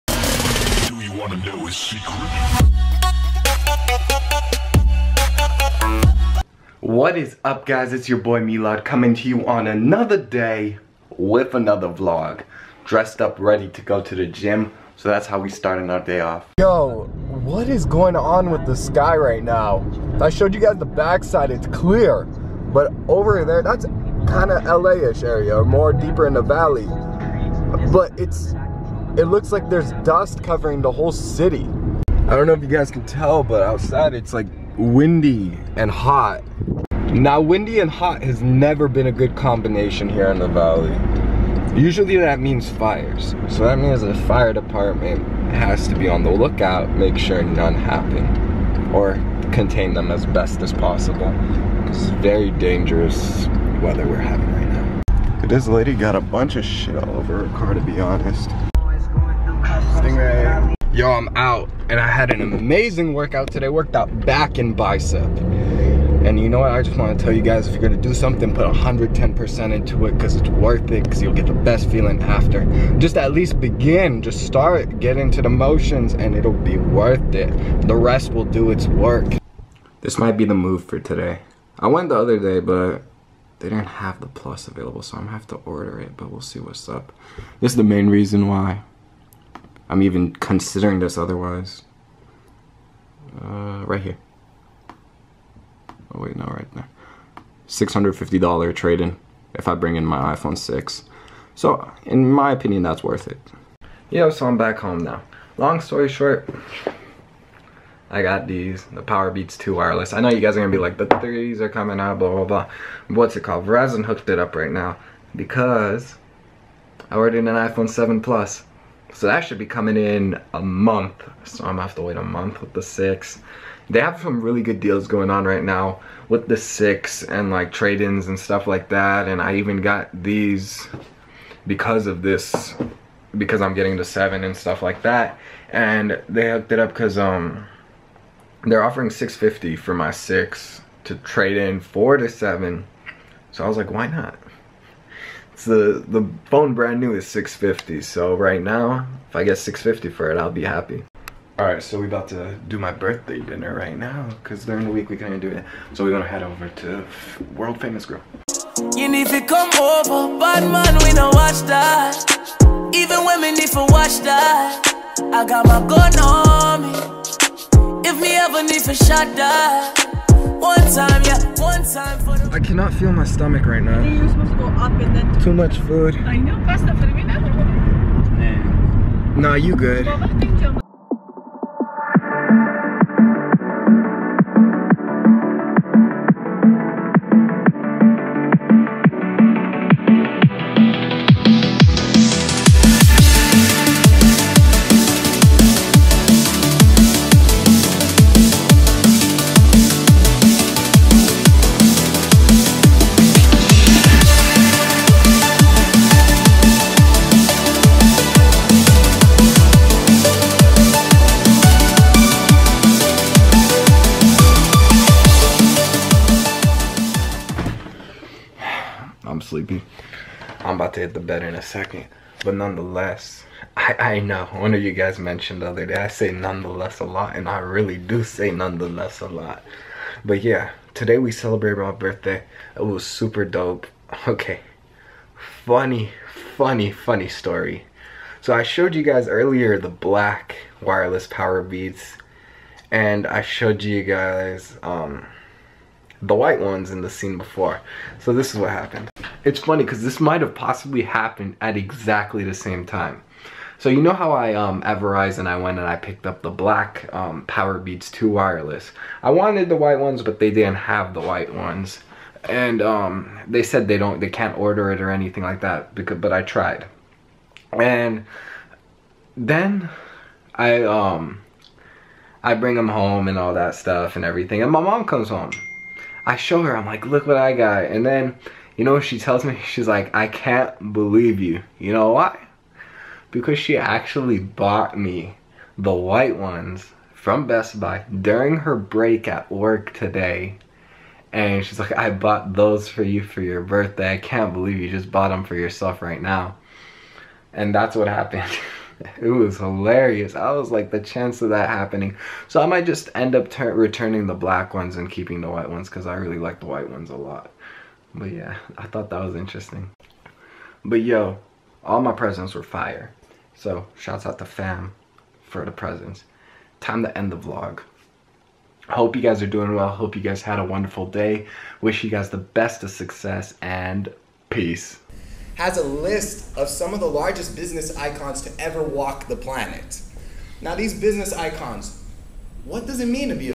CELEBRATING MY BIRTHDAY !!
What is up guys! Today was spent celebrating my birthday! Got some funny stories and some overall fun that went on today haha. Hope you all enjoy the vlog! :)

UBER CODE: melads